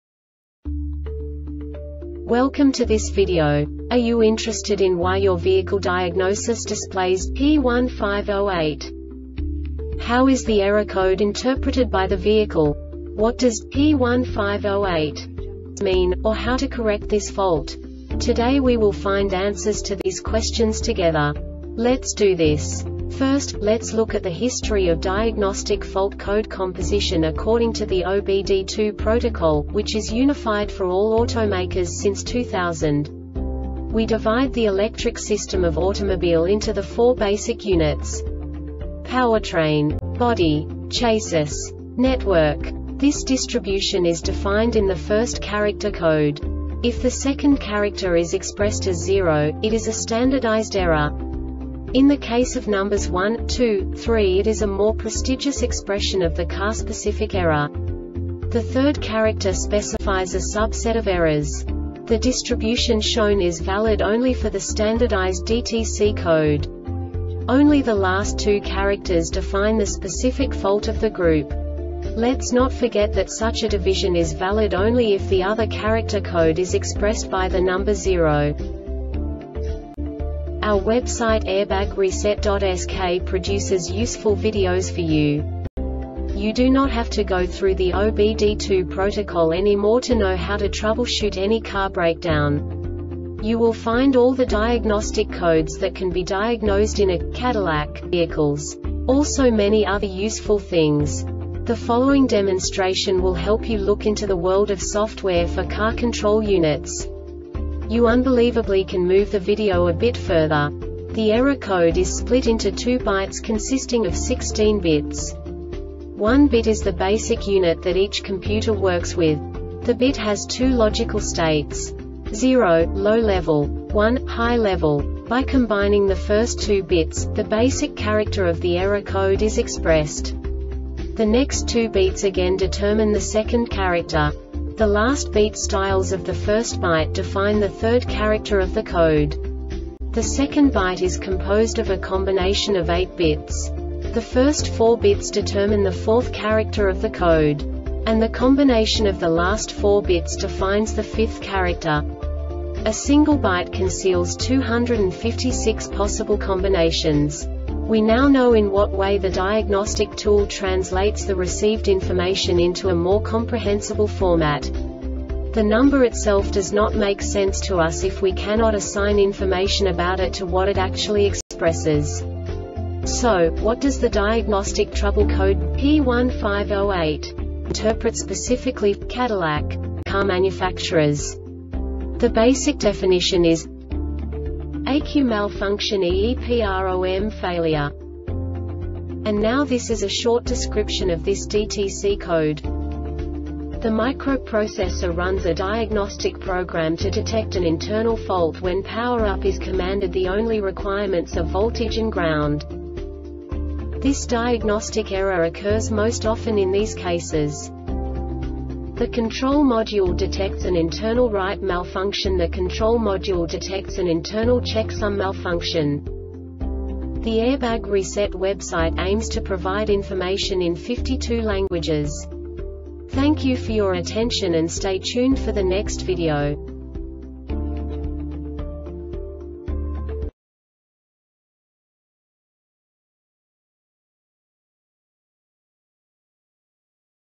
DTC cadillac P1508 Short Explanation
Contents:
0:21 Basic DTC analysis according to OBD2 protocol standard.
1:48 Insight into programming
2:58 A diagnostically understandable description of the Diagnostic Trouble Code
3:05 Basic error definition

Make:
Cadillac
Code:
P1508
Definition:
ECU Malfunction EEPROM Failure
Description:
The microprocessor runs a diagnostic program to detect an internal fault when power up is commanded.The only requirements are voltage and ground.
Cause:
The control module detects an internal write malfunction.The control module detects an internal checksum malfunction.